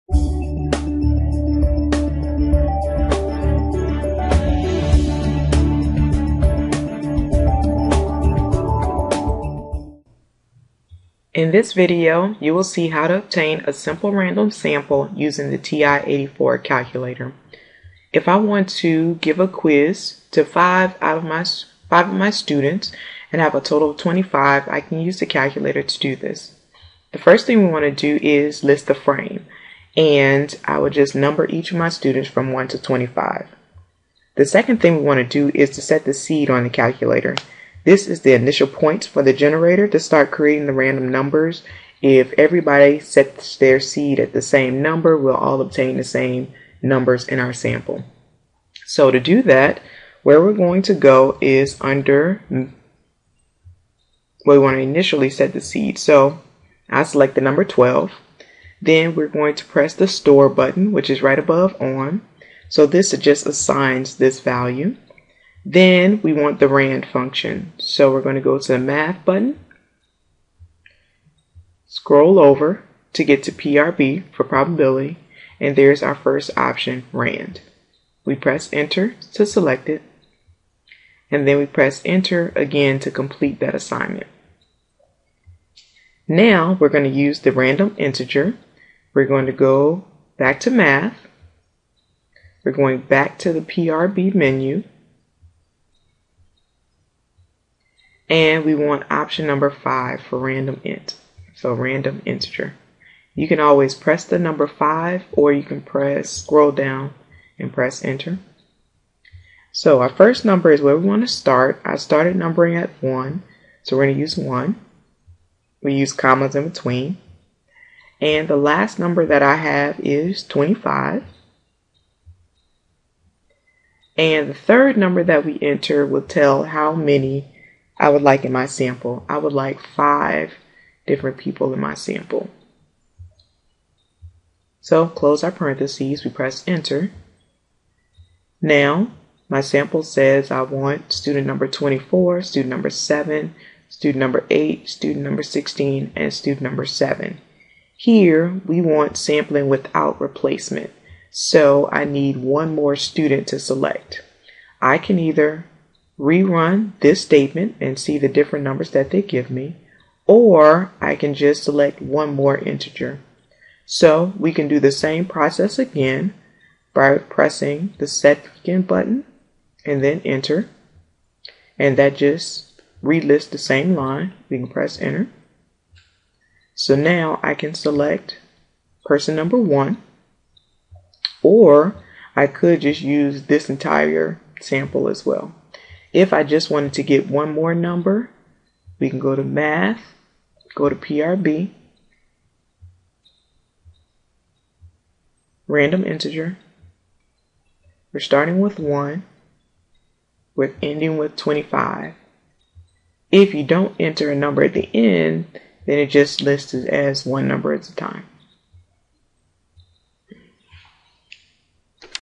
TI-84 Tutorial: How to Obtain a Simple Random Sample
**You can change the clarity of the video by changing your settings.  Click on the sprocket "wheel" under the video.**

In this video, you will see how to obtain a simple random sample using the TI-84 tutorial